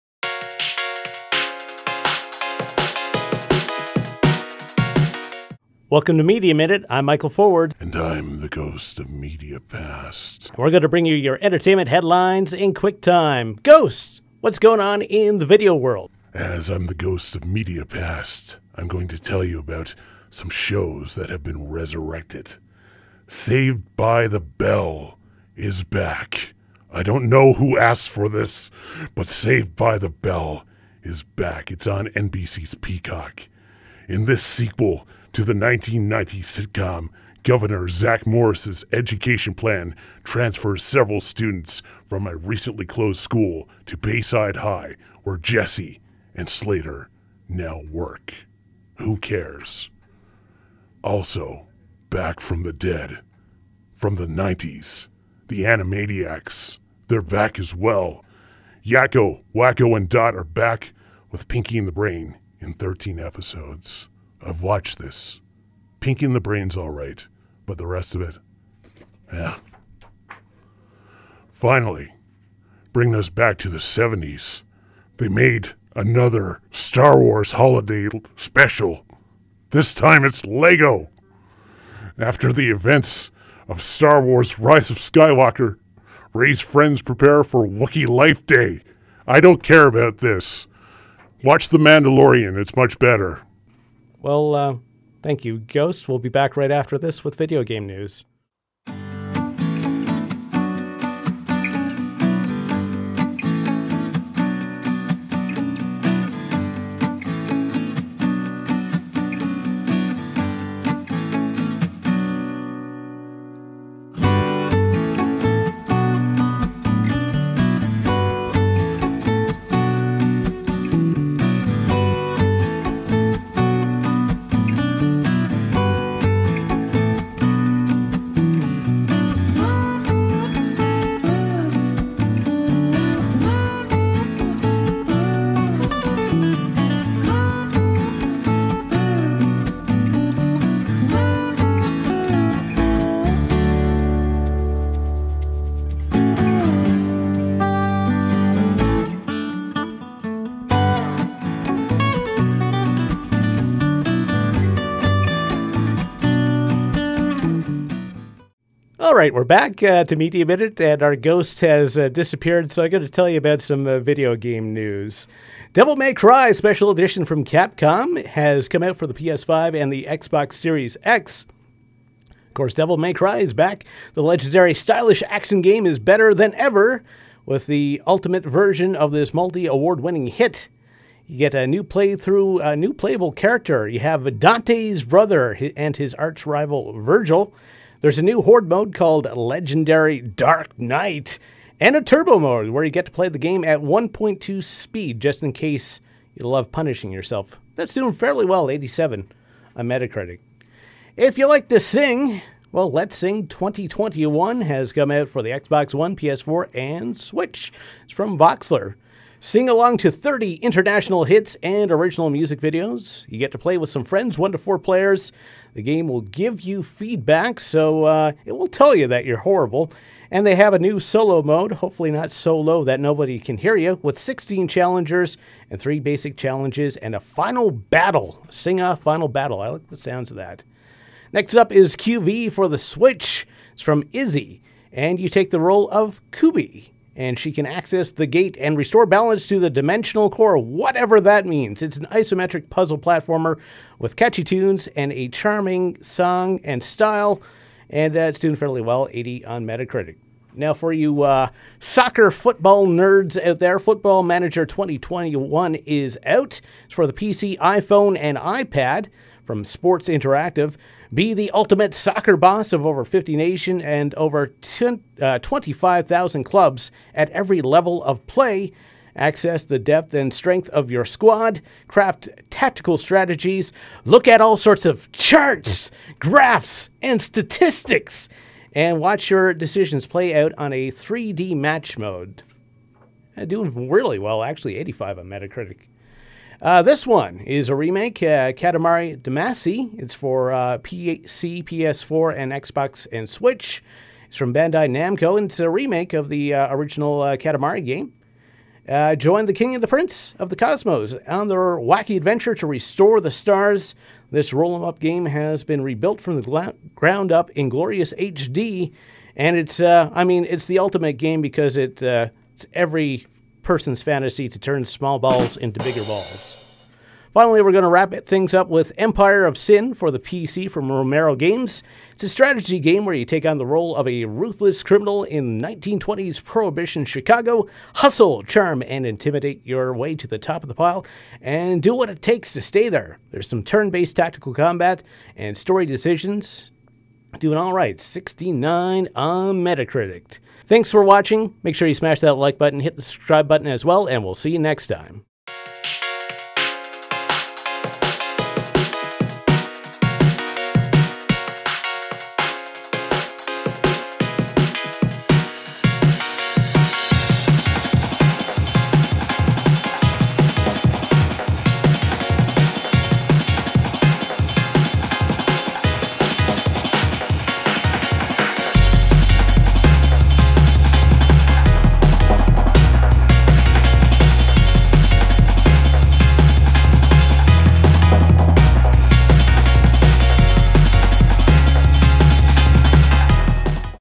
Media Minute: Episode 10
Welcome to Media Minute! Your entertainment headlines in quick times! In this week's episode, we have a guest host, the Ghost of Media Past and Mike lets us know about the latest games to hit the market!